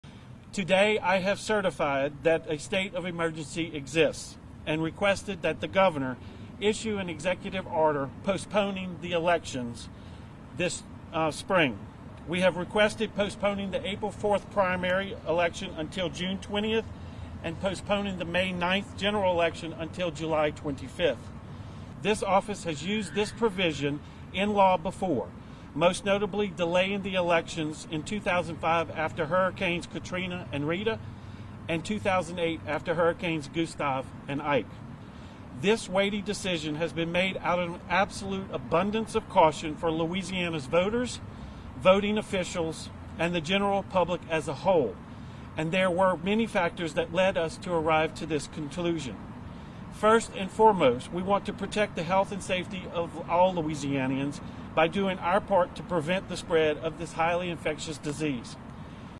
Louisiana delays primary vote due to virus concern
Louisiana officials Friday moved to delay presidential primary voting, out of concern for the spread of the COVID-19 virus in the state. Voting was set for April 4th, and a request was made for the governor to postpone the runoff to June 20. (March 13)